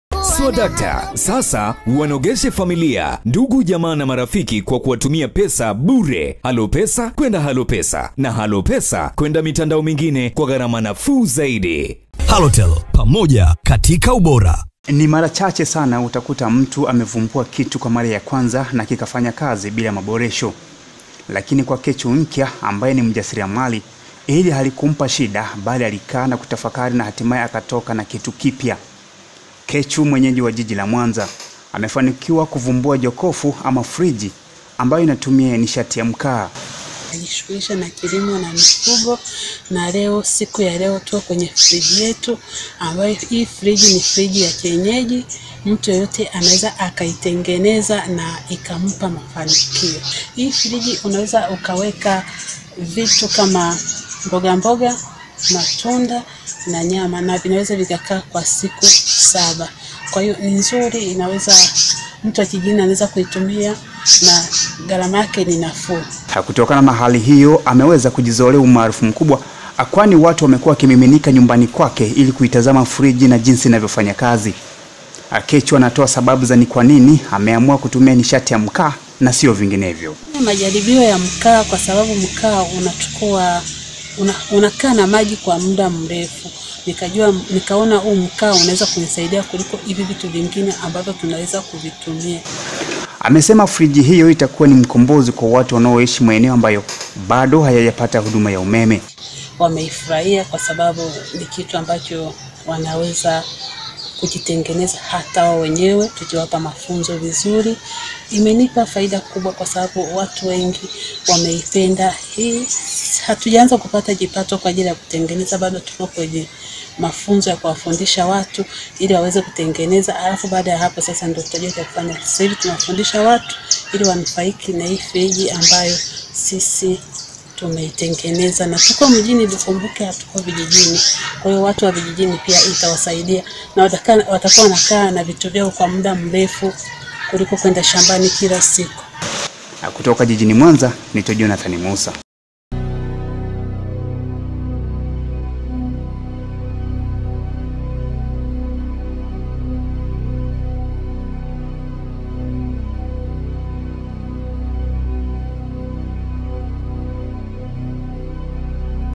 KUTANA NA MVUMBUZI WA JOKOFU (FRIJI) YA MKAA
Ni mara chache sana utakuta mtu amefumbua kitu kwa mara ya kwanza na kikafanya kazi bila maboresho.
Lakini kwa Kechu Nkya, ambaye ni mjasiriamali, hili halikumpa shida bali alikaa na kutafakari na hatimaye akatoka na kitu kipya.
Kechu mwenyeji wa jijini Mwanza, amefanikiwa kuvumbua jokofu ama friji ambayo inatumia nishati ya mkaa. Kutokana na hali hiyo, ameweza kujizolea umaarufu kwani watu wamekuwa wakimiminika nyumbani kwake ili kuitazama friji na jinsi inavyofanya kazi.